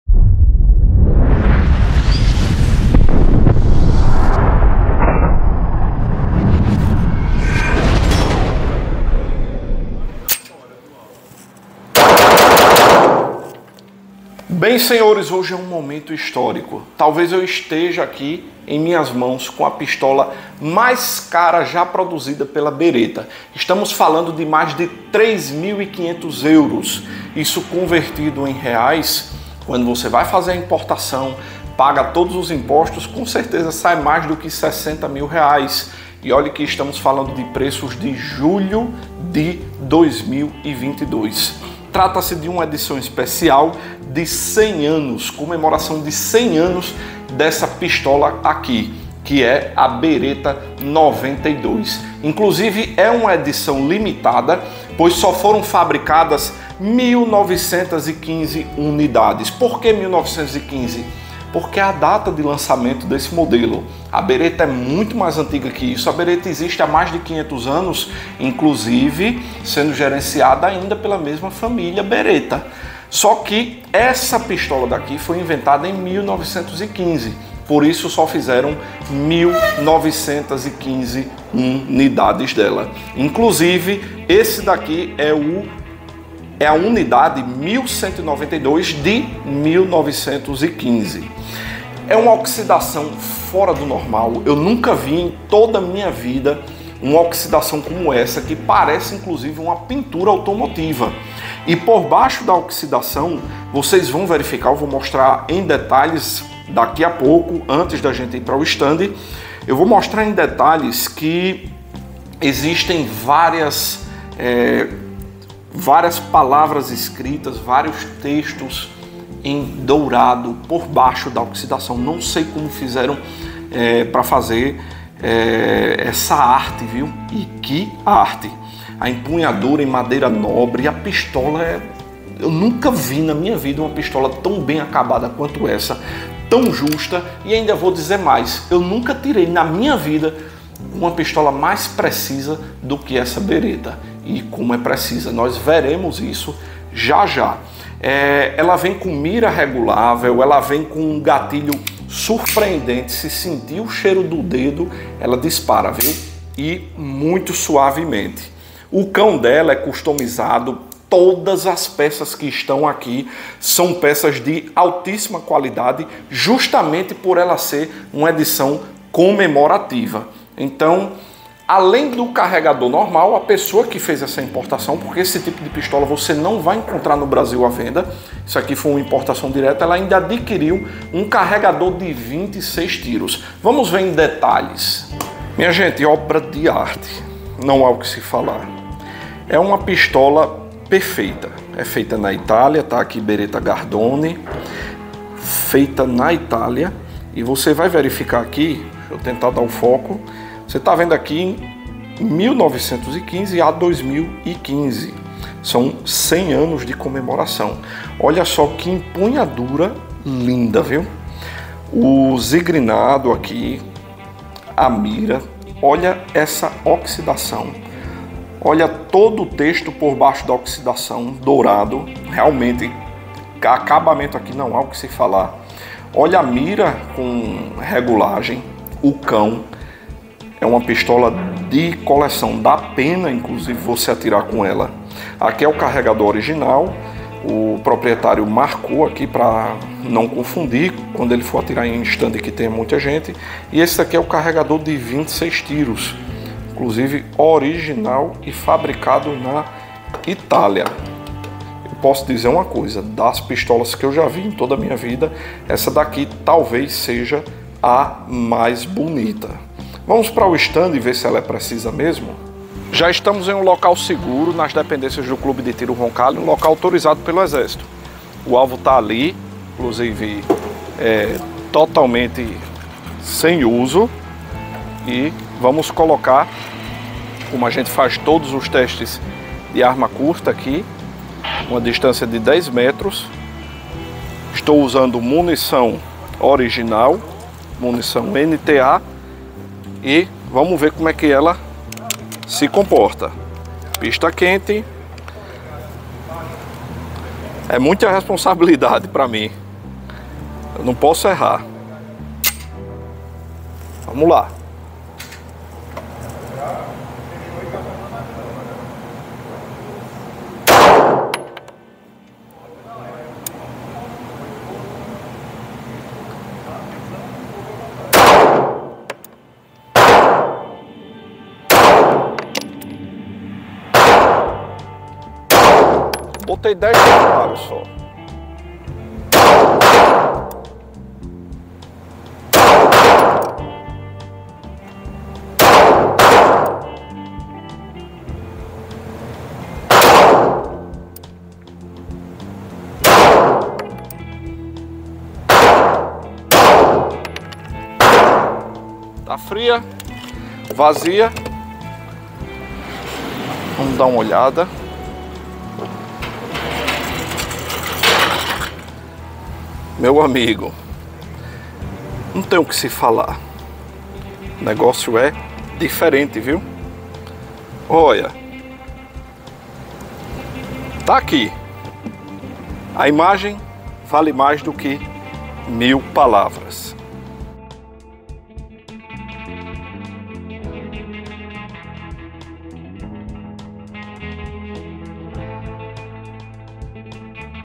Beretta 92 edição comemorativa de 100 anos - Review completo
Uma jóia de mais de 50 mil reais caso você deseje importar para o Brasil.